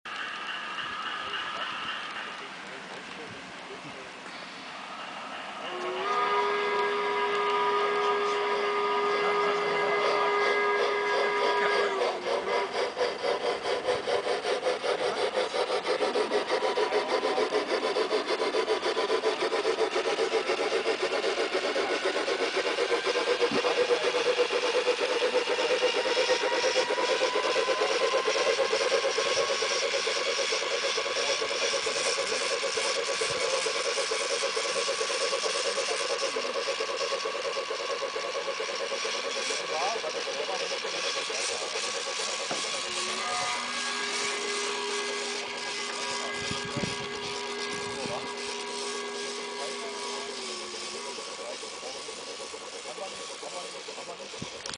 Coole Geräusche
So hört sich das Getriebe von Bachmann in der ersten Generation an.